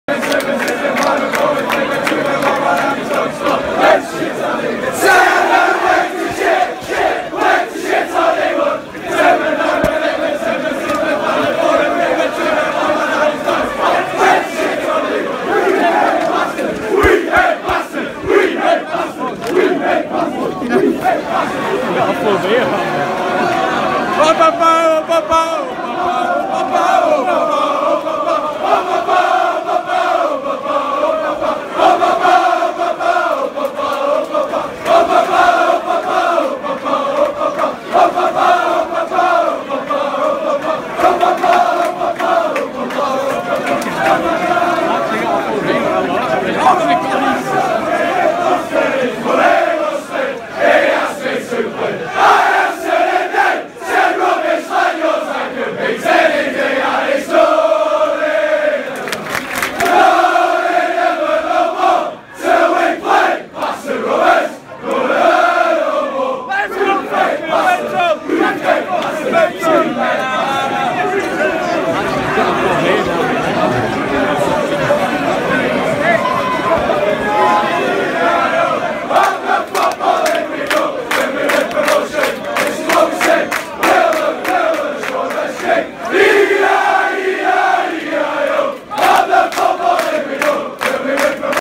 Burnley Fans at Blackburn 24th October 2015! Mental!!
Burnley fans at Blackburn

24th October 2015, Blackburn 0-1 Burnley